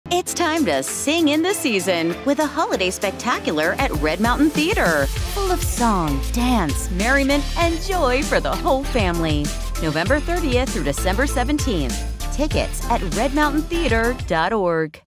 Holiday Spectacular 2023 | Now through December 17!
The Holiday Spectacular has jingled into Red Mountain Theatre! Join us now through December 17.